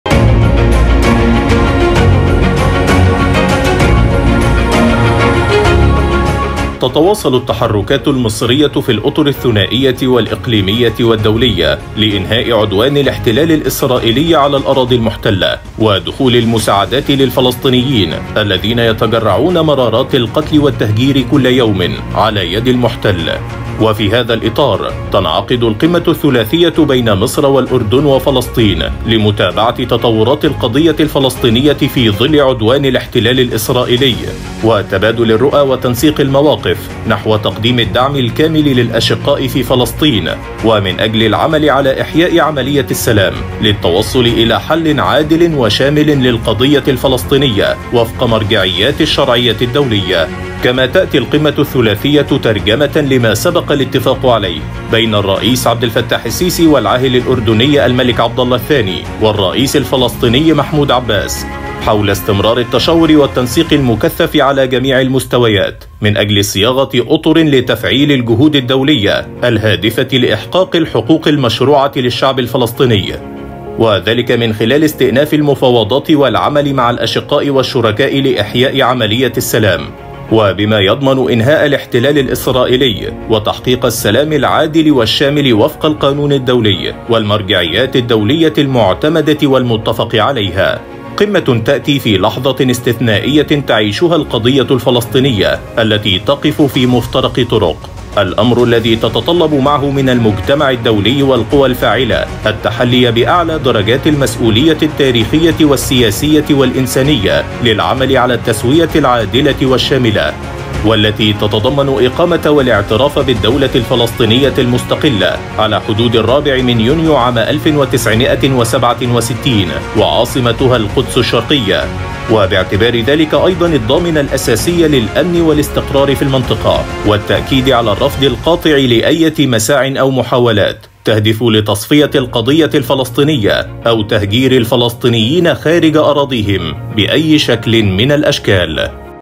قمة مصرية - أردنية - فلسطـ ينية حول مستجدات القضية الفلسطـ ينية
تغطية خاصة| قمة مصرية - أردنية - فلسطـ ينية حول مستجدات القضية الفلسطـ ينية

10 يناير 2023